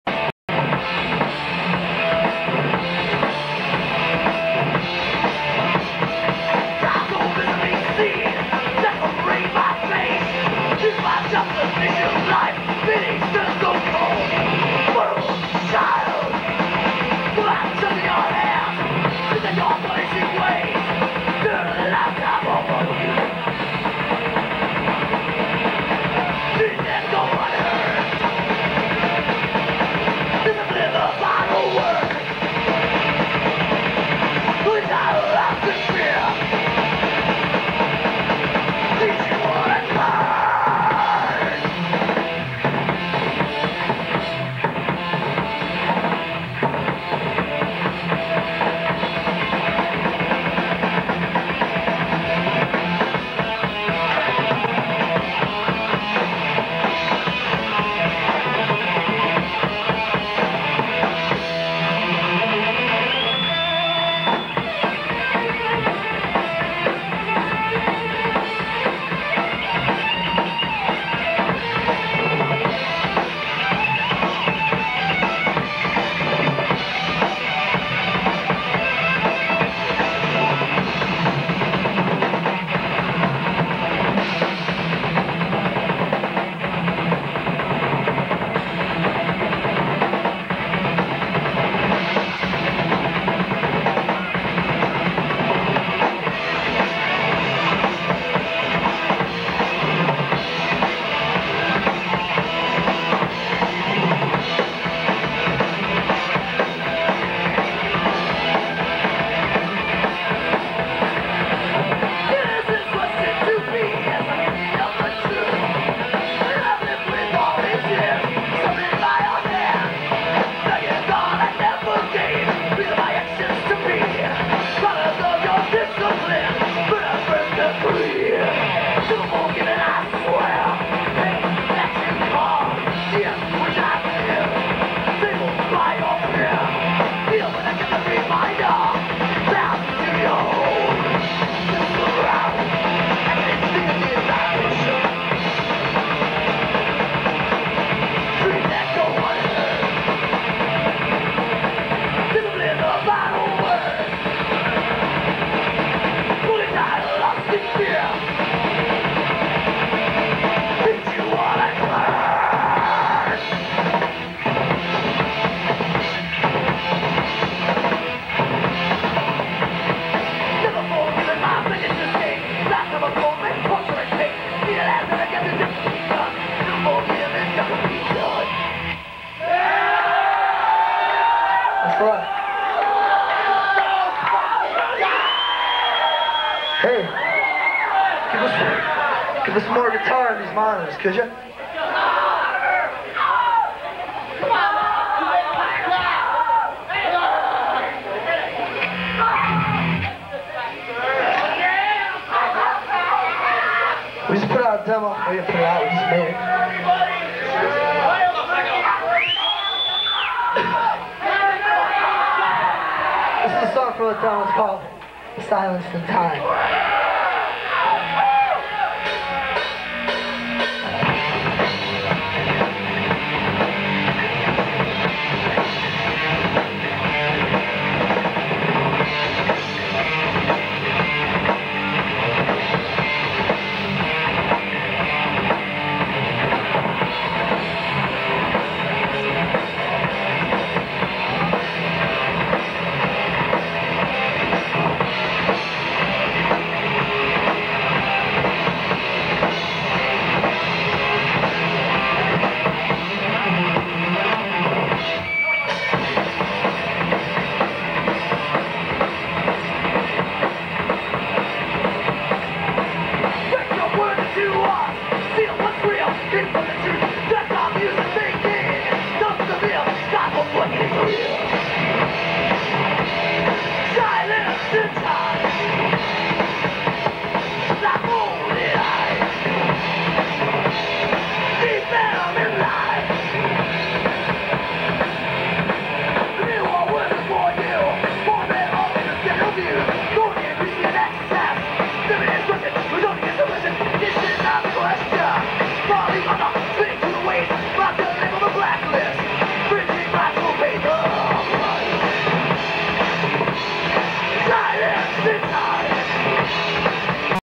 Decimation - Live At Flash Gordon's Night Club,Cleveland,Ohio,usa,17/08/1990
Thrash metal

Country : USA